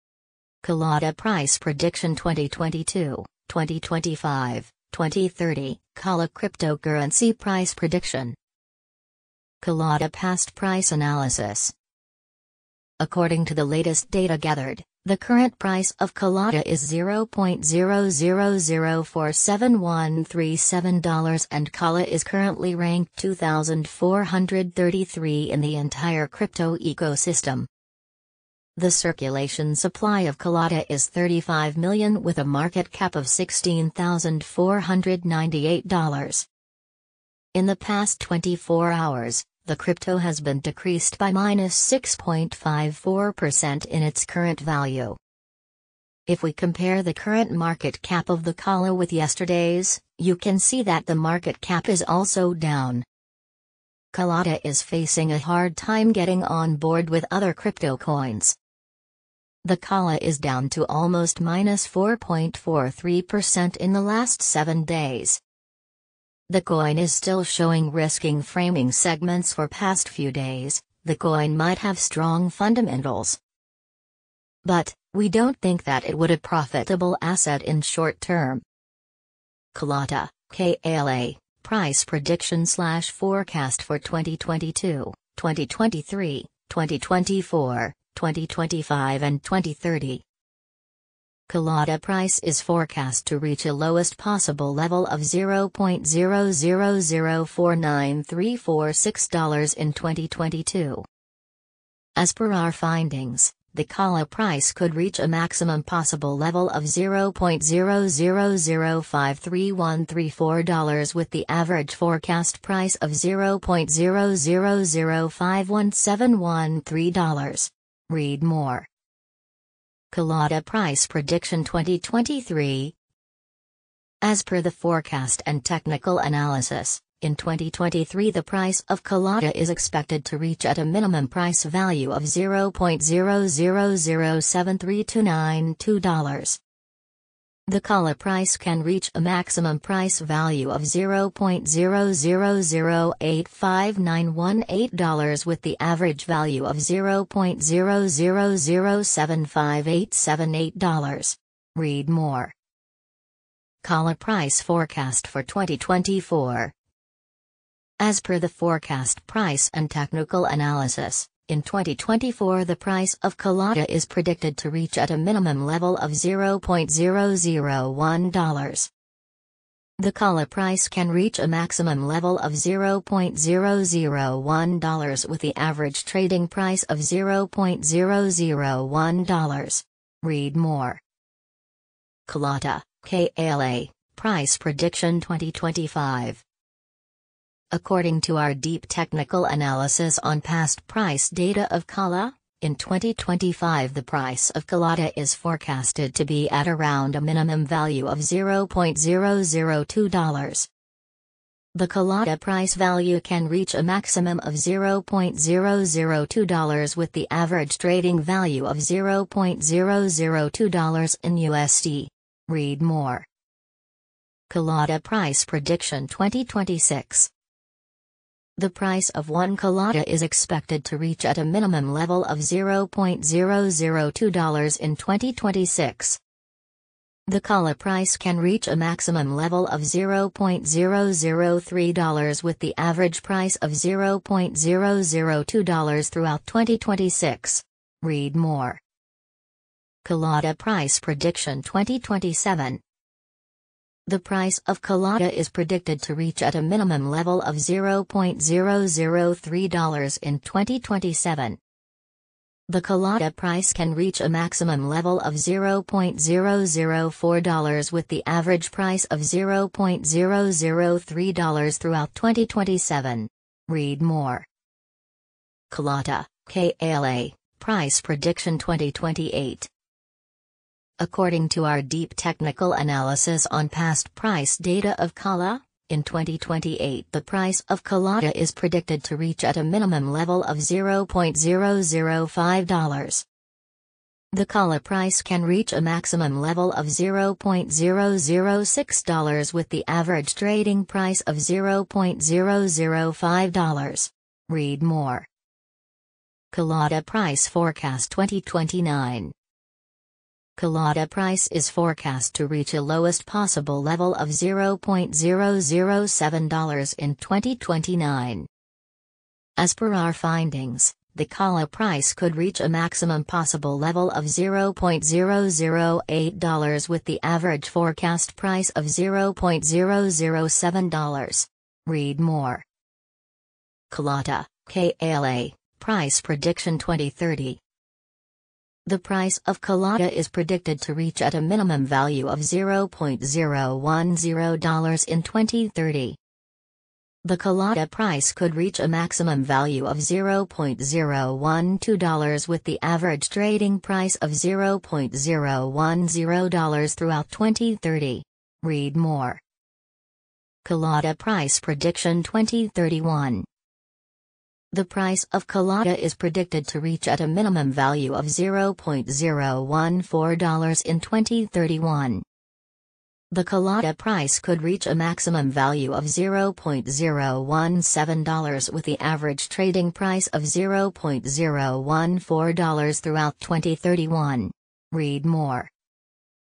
Kalata Price Prediction 2022, 2025, 2030 KALA Cryptocurrency Price Prediction
Kalata Past Price Analysis
According to the latest data gathered, the current price of Kalata is $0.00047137 and KALA is currently ranked 2433 in the entire crypto ecosystem.
The circulation supply of Kalata is 35,000,000 with a marketcap of $16,498.
If we compare the current market cap of the KALA with yesterday’s, you can see that the market cap is also down.
Kalata is facing a hard time getting on board with other crypto coins.
The KALA is down to almost -4.43% in the last 7 days.
Kalata (KALA) Price Prediction/Forecast for 2022, 2023, 2024, 2025 and 2030
Kalata price is forecast to reach a lowest possible level of $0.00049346 in 2022.
Kalata Price Prediction 2023
As per the forecast and technical analysis, In 2023 the price of Kalata is expected to reach at a minimum price value of $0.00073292.
KALA Price Forecast for 2024
As per the forecast price and technical analysis, In 2024 the price of Kalata is predicted to reach at a minimum level of $0.001.
Kalata (KALA) Price Prediction 2025
According to our deep technical analysis on past price data of KALA, In 2025 the price of Kalata is forecasted to be at around a minimum value of $0.002.
The Kalata price value can reach a maximum of $0.002 with the average trading value of $0.002 in USD.
Kalata Price Prediction 2026
The price of 1 Kalata is expected to reach at a minimum level of $0.002 in 2026.
The KALA price can reach a maximum level of $0.003 with the average price of $0.002 throughout 2026.
Kalata Price Prediction 2027
The price of Kalata is predicted to reach at a minimum level of $0.003 in 2027.
The Kalata price can reach a maximum level of $0.004 with the average price of $0.003 throughout 2027.
Kalata (KALA) Price Prediction 2028
According to our deep technical analysis on past price data of KALA, In 2028 the price of Kalata is predicted to reach at a minimum level of $0.005.
Kalata Price Forecast 2029
Kalata price is forecast to reach a lowest possible level of $0.007 in 2029.
Kalata (KALA) Price Prediction 2030
The price of Kalata is predicted to reach at a minimum value of $0.010 in 2030.
The Kalata price could reach a maximum value of $0.012 with the average trading price of $0.010 throughout 2030.
Kalata Price Prediction 2031
The price of Kalata is predicted to reach at a minimum value of $0.014 in 2031.
The Kalata price could reach a maximum value of $0.017 with the average trading price of $0.014 throughout 2031.